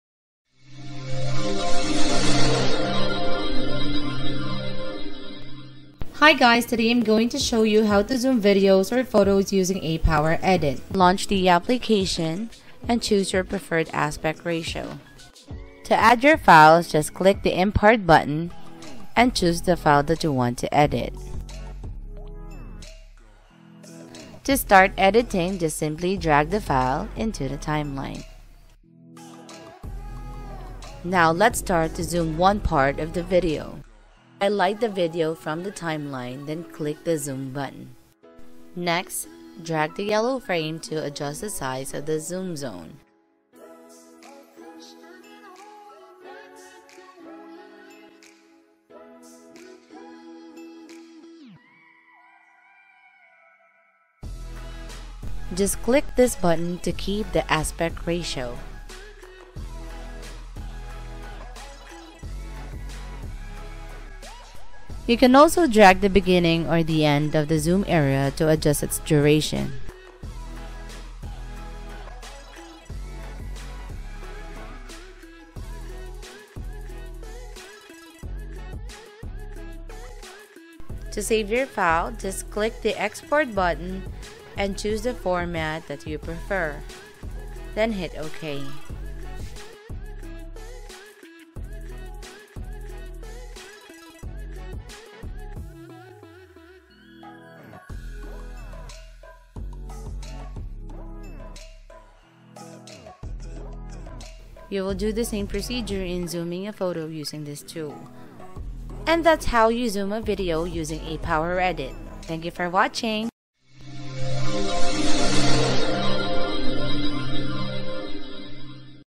How to zoom videos
Learn how to Zoom your videos using ApowerEdit.
It is the best video editing software!